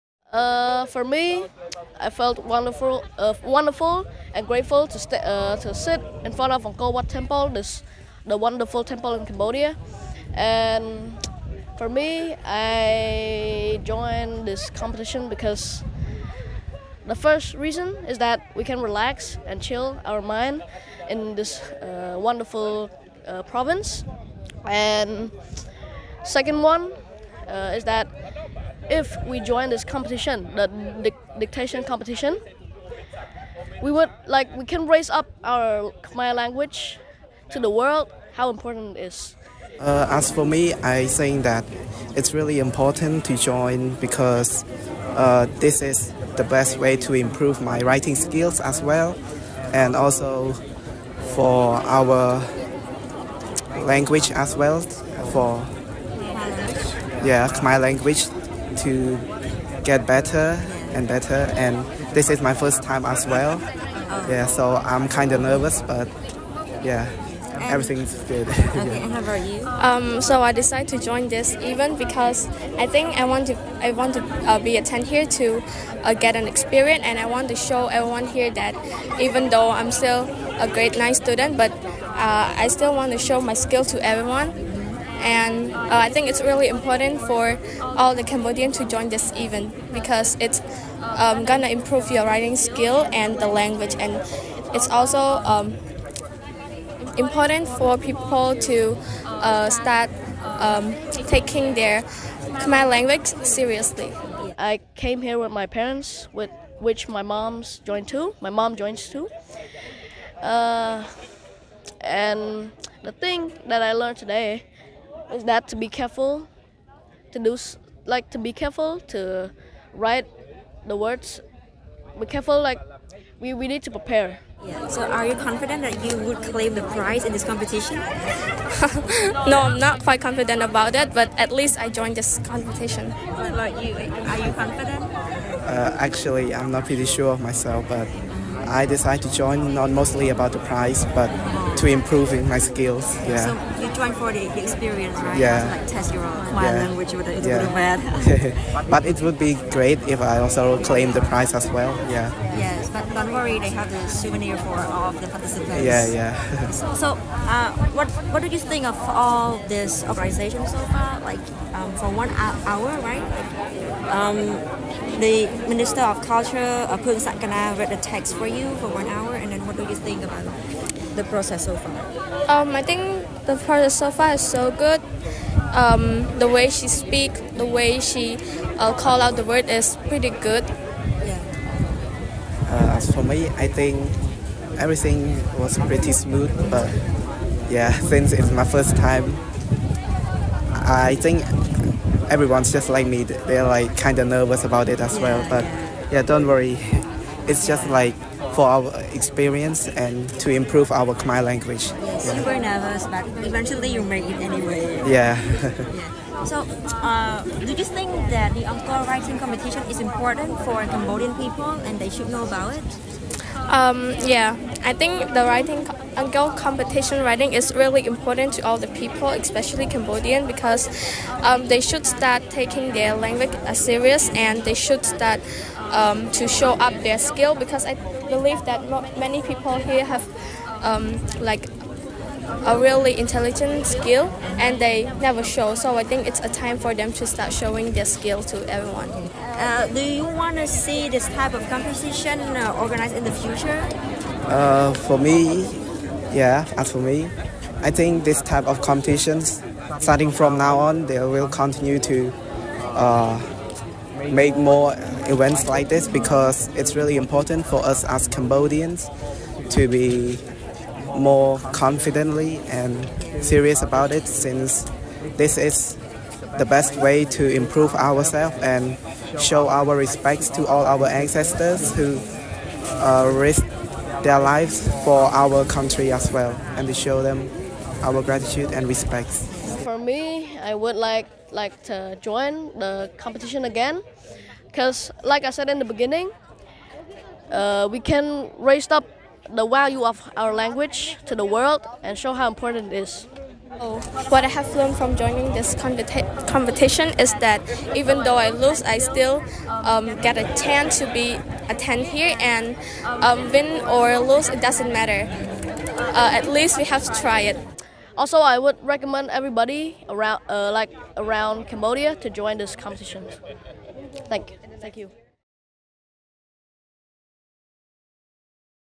The Angkor Writing: young participants hope to see this competition become an annual event
Gaining experience, testing their Khmer language skills and being part of promoting the language were the aims of the participants in the "Angkor Writing" competition held at the Angkor Wat causeway bridge in the early morning of Jan. 21.

Roughly 700 people flocked to Siem Reap province to compete in the event, which was the first of its kind, the results to be announced on the same day at 4 pm.

The young participants hope to see this competition become an annual event so that the Khmer language can be further promoted, and Cambodians can be more aware of their own language and heritage.